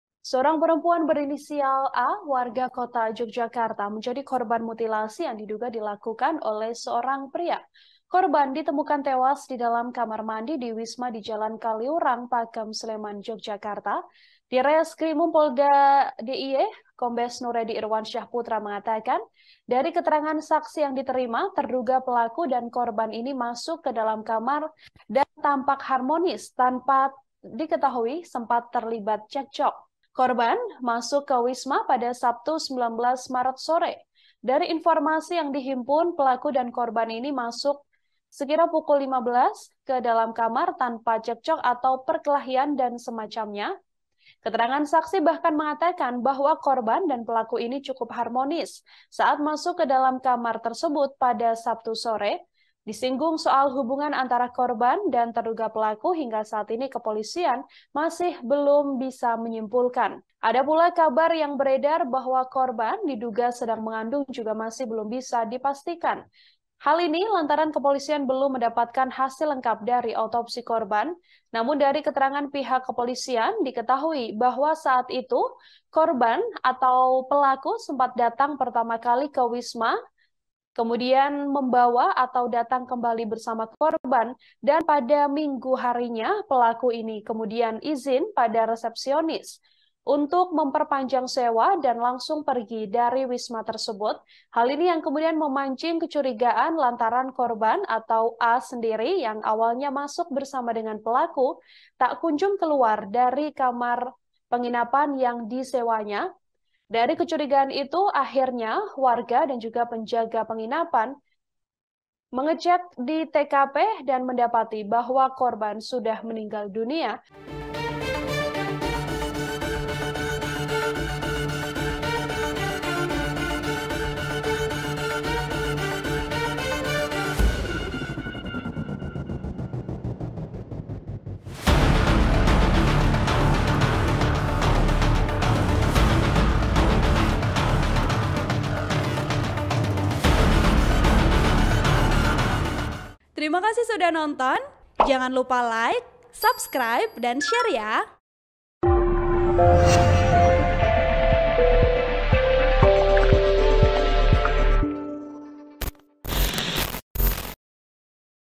Saksi Ceritakan Detik-detik Korban Mutilasi Sleman Masuk dengan Pelaku ke Penginapan, Awalnya Baik
TRIBUN-VIDEO.COM - A (35) warga Patehan, Kota Jogja ditemukan di sebuah wisma penginapan di padukuhan Purwodadi, Pakembinangun Pakem Kabupaten Sleman.

Mayat tersebut ditemukan oleh Penjaga wisma penginapan curiga seusai tamu yang menginap sejak Sabtu (18/3/2023) sore tak juga keluar kamar.

Pria terduga pelaku datang ke salah satu wisma bersama korban. Keduanya tampak harmonis saat masuk ke dalam kamar.